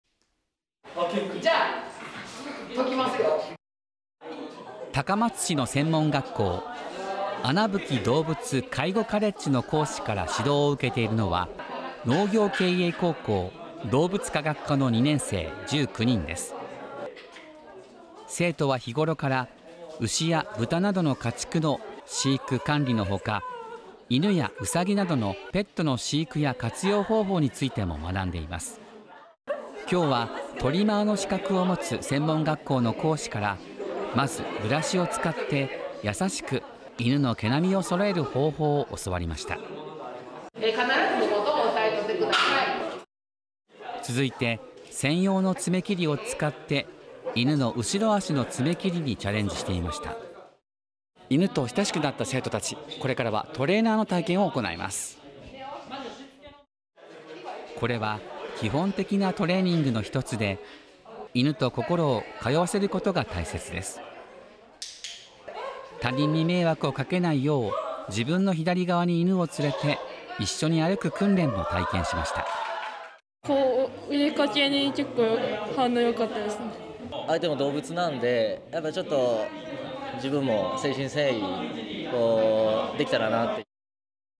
高校生が犬のしつけやトリミングに挑戦 動物の気持ちや扱い方学ぶ 香川
家畜の飼育・管理法などを学んでいる香川県の高校生が犬のしつけやトリミングを通して動物の気持ちや扱い方を学びました。